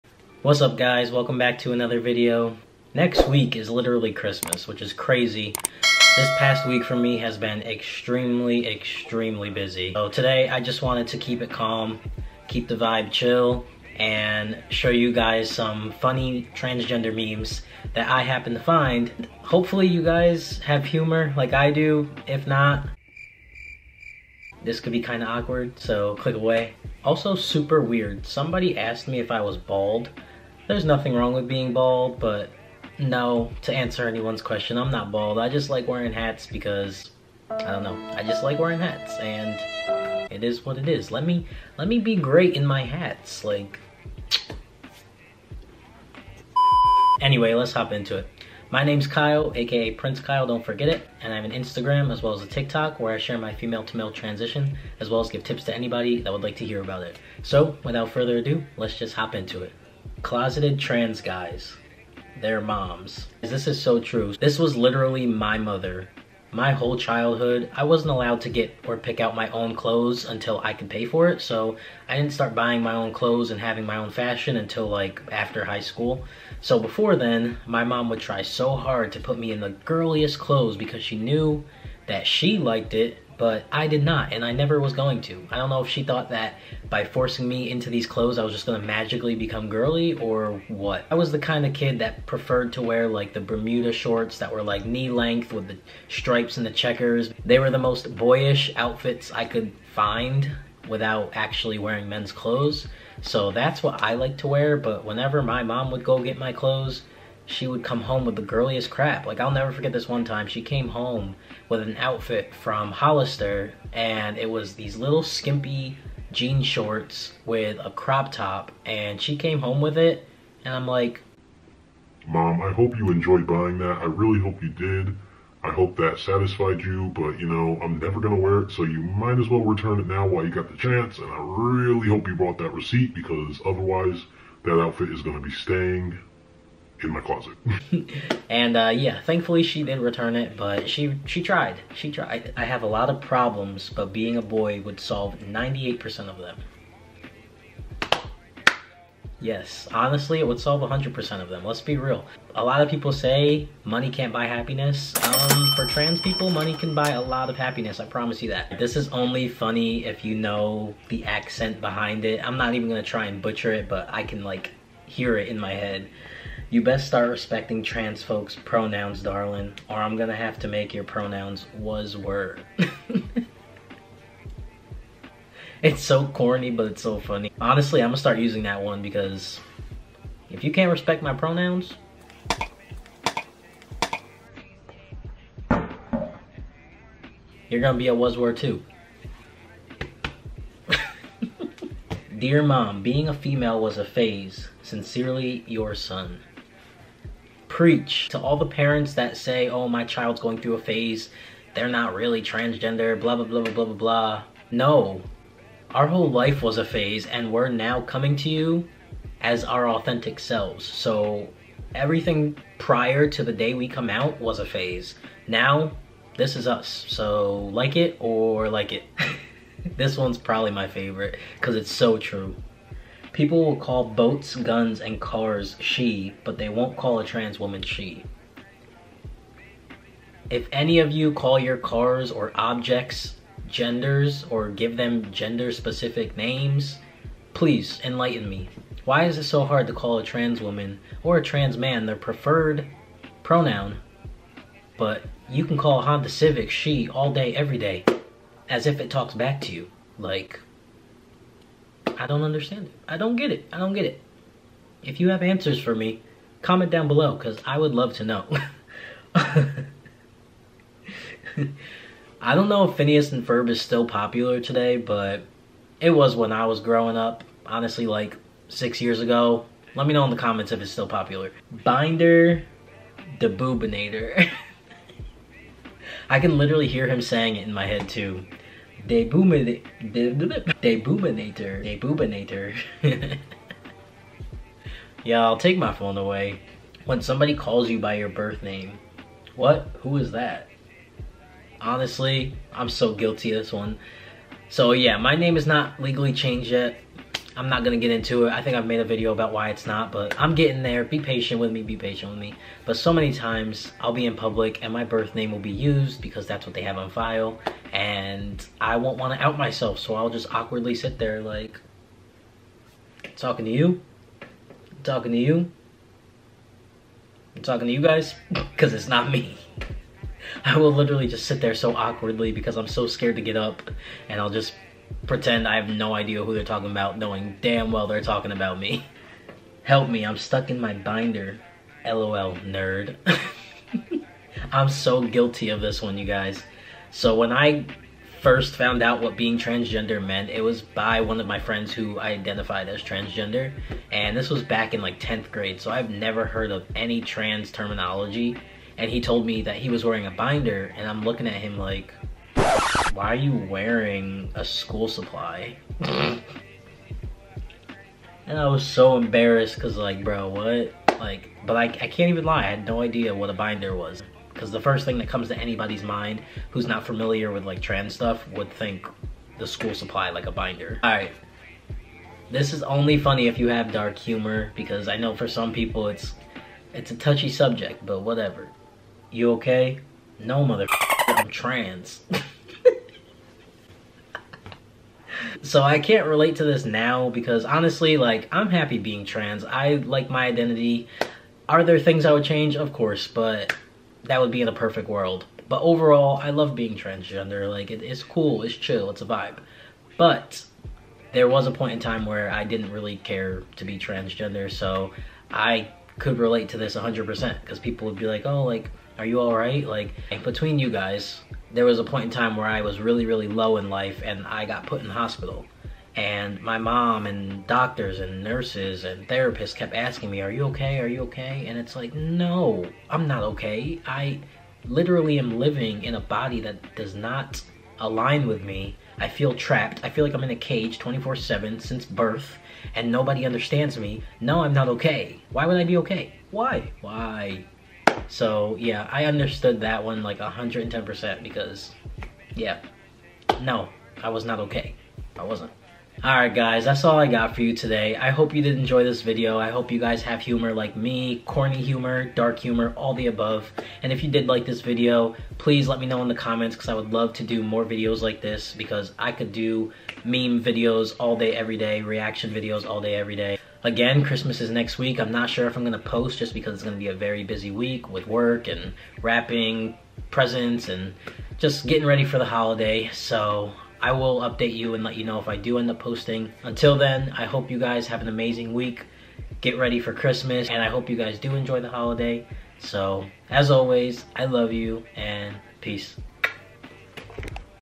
TRY NOT TO LAUGH, TRANSGENDER MEMES
Try not to laugh!! (Dank Memes)
Searching for my sense of humor crowd 😂🏳️‍⚧️

Found some funny memes I wanted to share, hope y'all enjoy. Let me know down in the comments which meme was your favorite!
Road to 50K thanks to all of you once we reach that ill be doing another give away!

FTM Memes Reaction dank memes